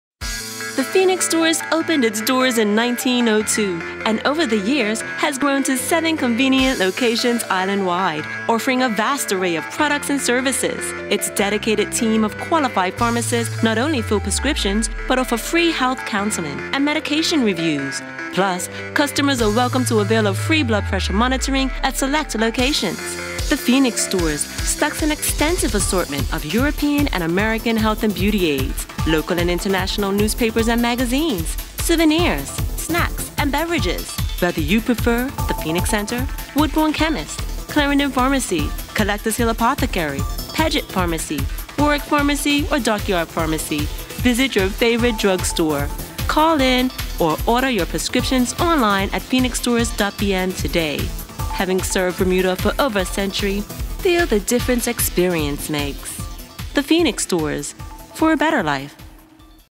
The Phoenix Stores - For A Better Life
The Phoenix Stores was established in 1902 and after operating for over 100 years the company is renowned for delivering quality products and outstanding service in a variety of retail divisions.

Amongst The Phoenix Stores is a diverse range of products including: Health and Beauty, Home Décor, Perfumes and Fragrances, Designer Sunglasses, Fashion Jewellery and Accessories, Consumer Electronics, Photo Development, Toys and Children’s Health and Safety, Souvenirs, Greeting Cards, Party Supplies, Books and Newsstand Publications.